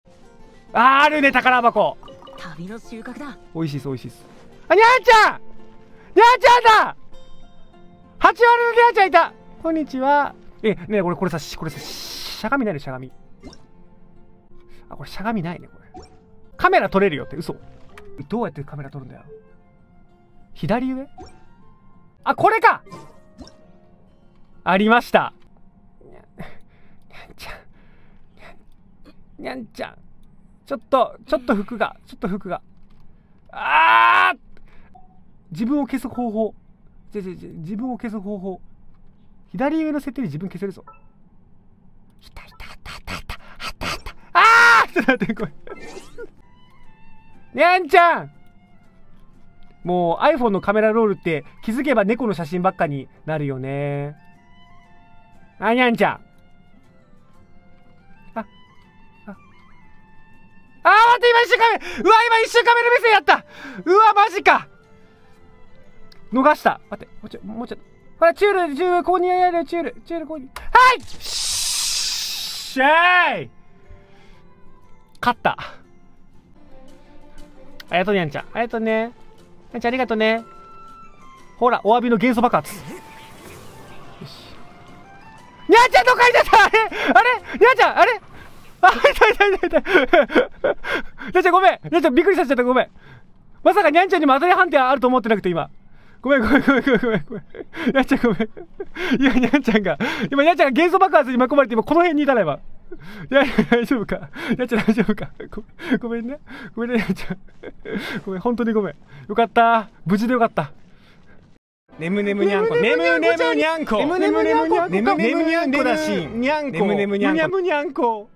akina attacks a cat with a tornado | Nijisanji eng subs
Akina: @SaegusaAkina

Feel free to correct me if you see any mistakes!
もし間違えていたら遠慮なく直して下さい！

0:00 nyan-chan appears
1:21 nyan-chan obliterated